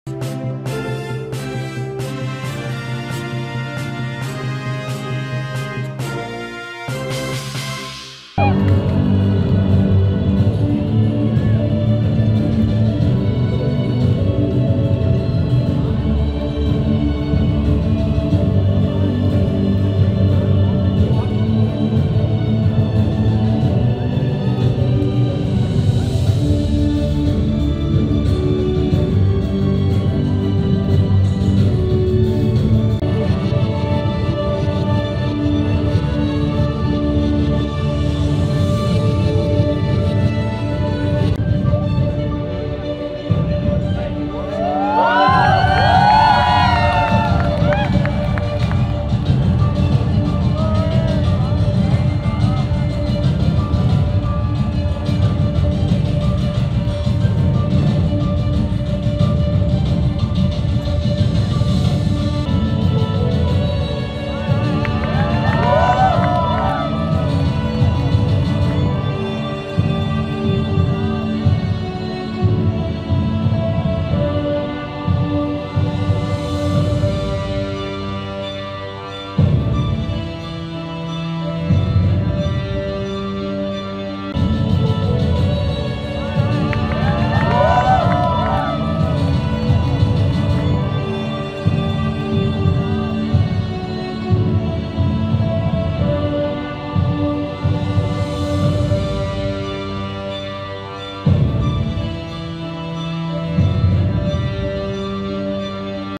અદ્‌ભૂત ડ્રોન નજારો અમદાવાદમાં જબરદસ્ત ડ્રોન શો
નેશનલ ગેમ્સના ખેલાડીઓના સ્વાગત માટે અમદાવાદમાં જબરદસ્ત ડ્રોન શો

narendramodi nationalgames ahmedabadnews amdavad riverfront દેશ-વિદેશની મહત્વપૂર્ણ ખબરો આપણી પોતાની ભાષા ગુજરાતી માં ADIKHAM GUJARAT (અડિખમ ગુજરાત) ગુજરાતી news પર